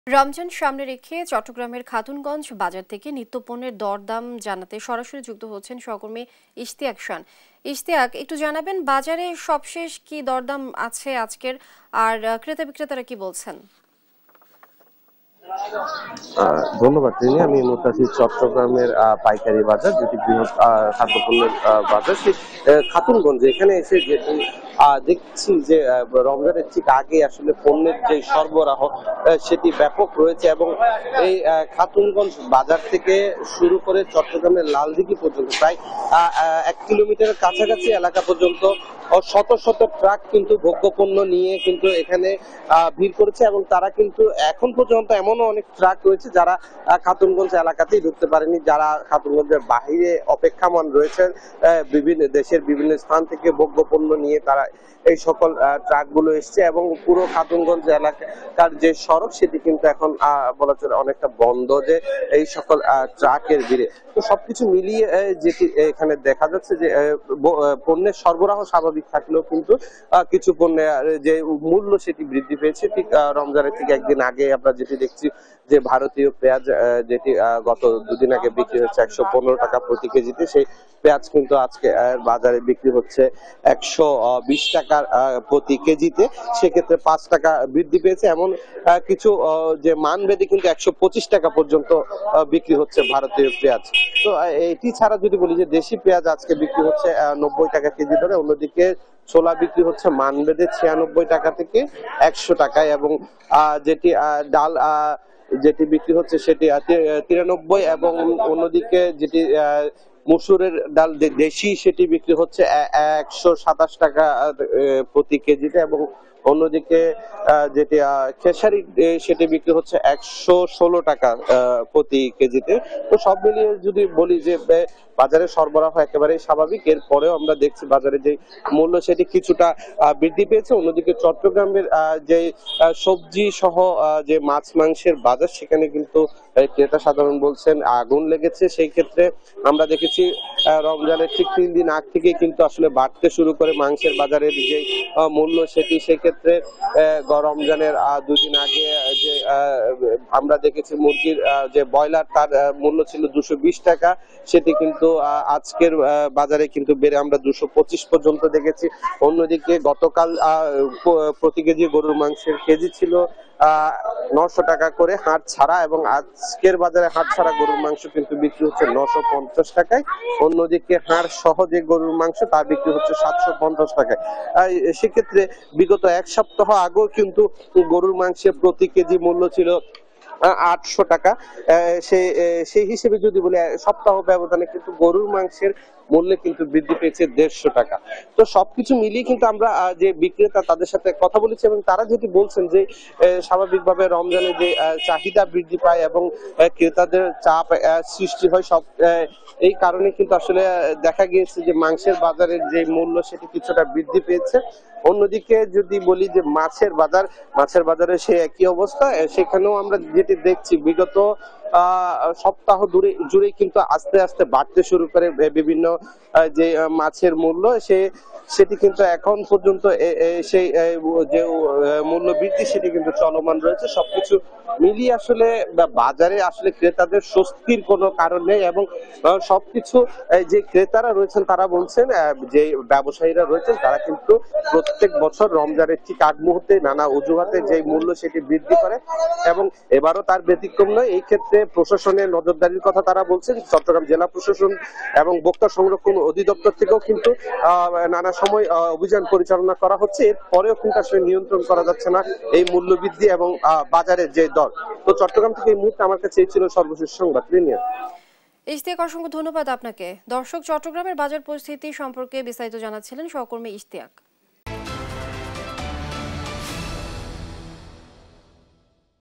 বাজার: মজুদ-সরবরাহ স্বাভাবিক তবু বেশি দামে বিক্রি | Chattogram | Bazar Update | News | Desh TV
»» Office Address:
42, Shaheed Sangbadik Selina Parveen Sarak, Malibag 1217 Dhaka, Dhaka Division, Bangladesh